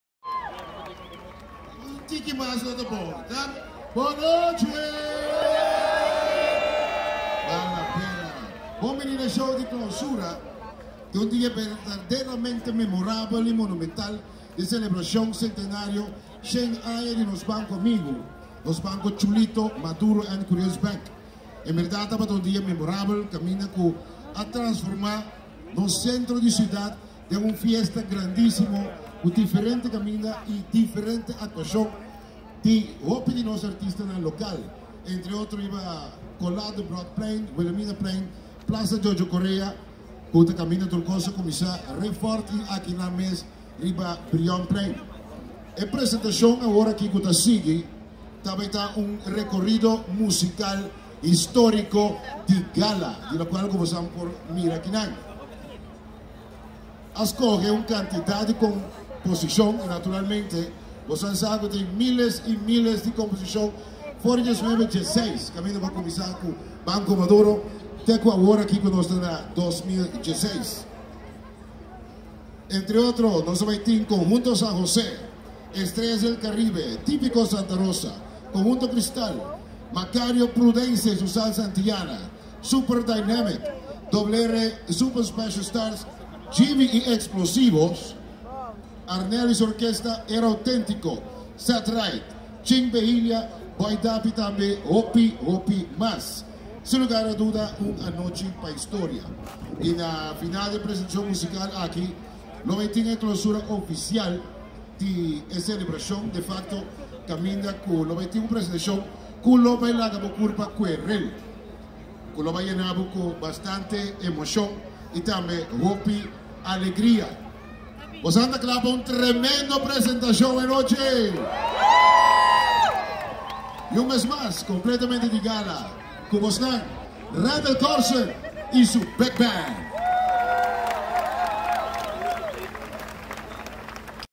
Randal Corsen and Friends 01, 100 aña di MCB 2016
Randal Corsen; Vocals: Yumarya Grijt, Rich Peter, Gilbert Dibo Doran, Izaline Calister, Area 51; Band: Igort Rivas, Eric Calmes, Eric Brigitha, Pernell Saturnini, Jean-Jacques Rojer, Royston Hato, Hector Sampson, Marlon Conradus, Ismael Gonzalez Lopez, Hershel Rosario, Ramon Martinez, Ernie Gregorius and more; Sound: Oscar Bor.
At Curaçao, Otrobanda, Plasa di Brión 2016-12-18.
Captured by Joscelin Trouwborst with Lumia 950 XL.